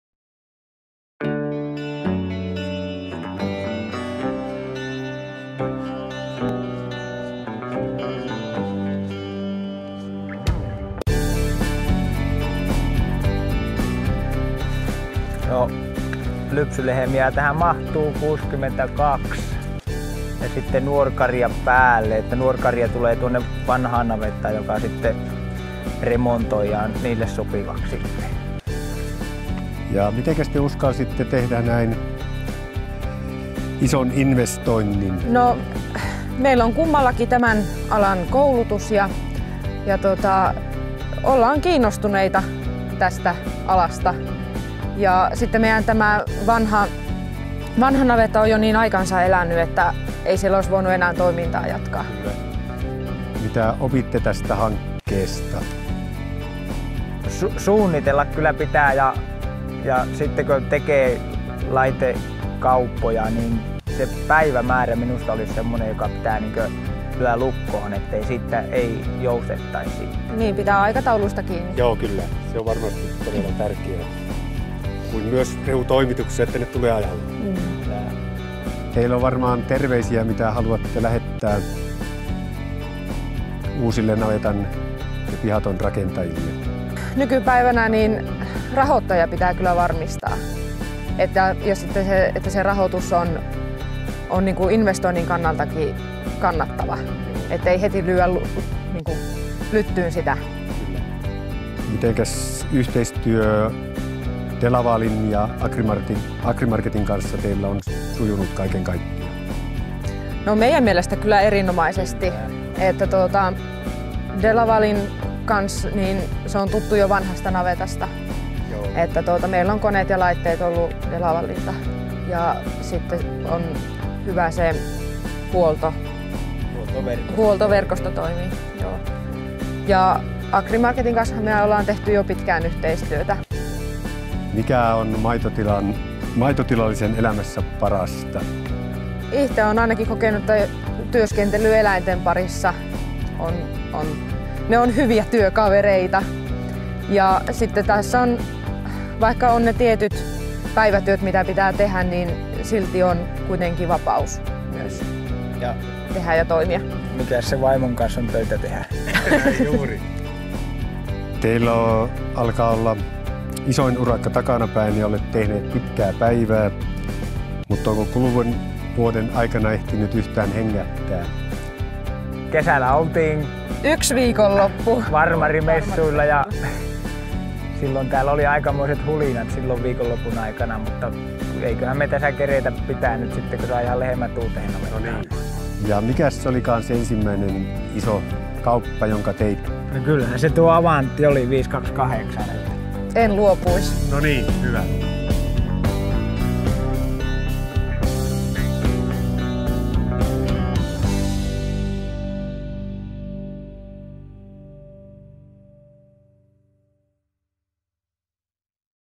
Muuraiskankaan tilan uuden pihattonavetan esittely
Muuraiskankaan tilan uuden pihaton avajaiset Kälviällä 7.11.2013.